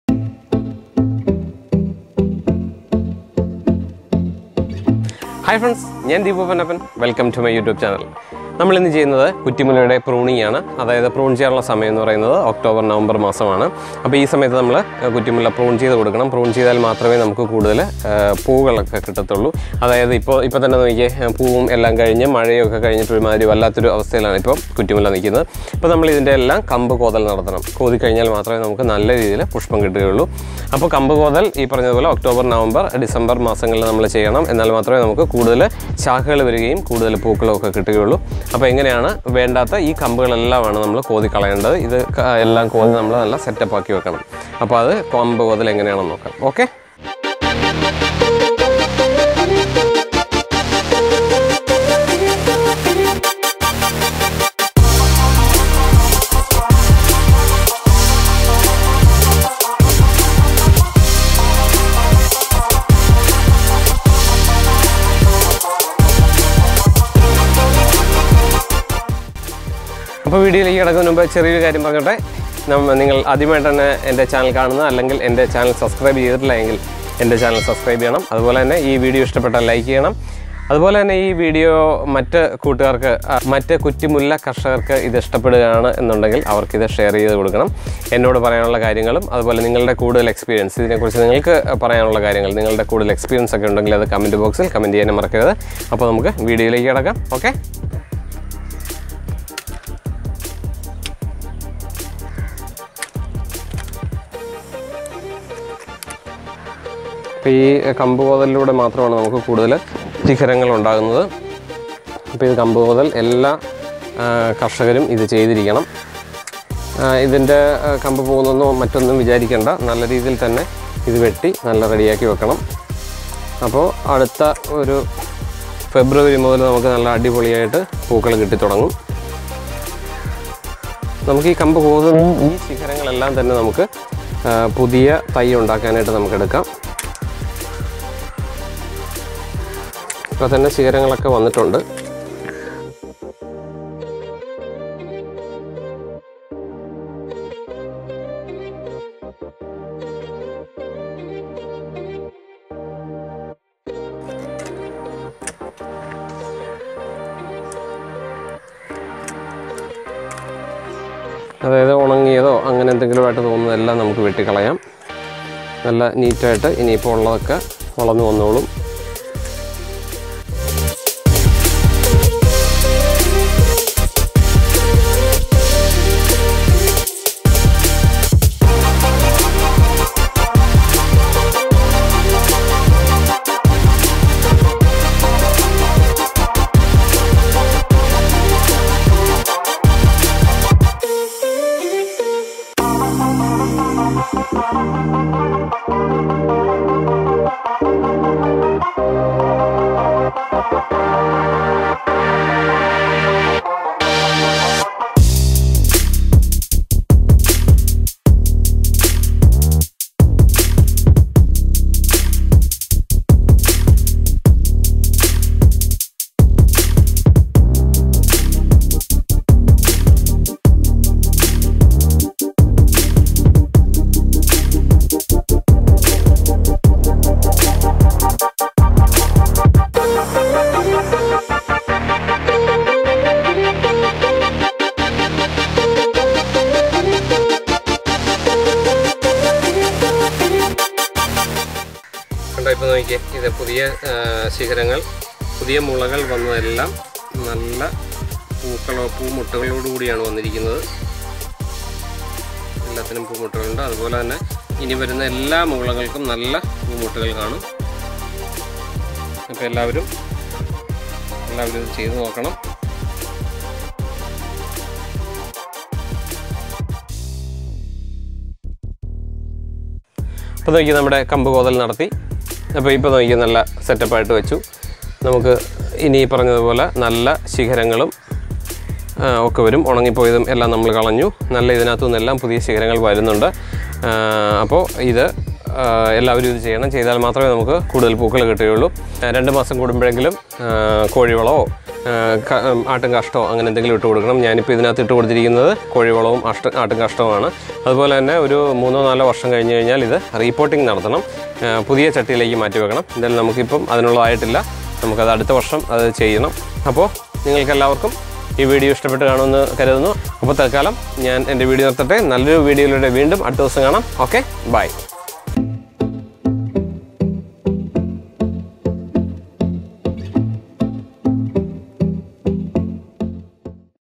Jasmine Cultivation in Kerala | Jasmine Pruning | കുറ്റിമുല്ലയുടെ കൊമ്പ് കൊതൽ | Deepu Ponnappan
കുറ്റിമുല്ലകൃഷിയിൽ ഏറ്റവും പ്രധാനപ്പെട്ട ഒന്നാണ് കുറ്റിമുല്ലയുടെ കൊമ്പ് കൊതൽ. ഇത് എങ്ങനെ, എപ്പോൾ തുടങ്ങിയ എല്ലാ വിവരങ്ങളും ഈ വിഡിയോയിൽ ഉൾപ്പെടുത്തിയിട്ടുണ്ട്.